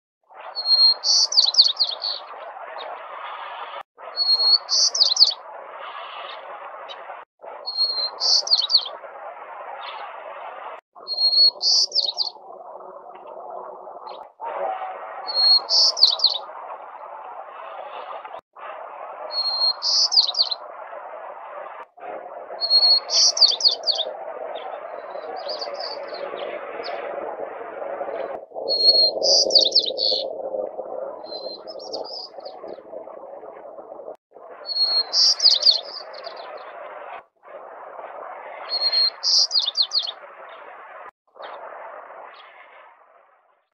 WCSP 20201102
White-crowned Sparrow singing in November in Cesar Chavez Park, Berkeley California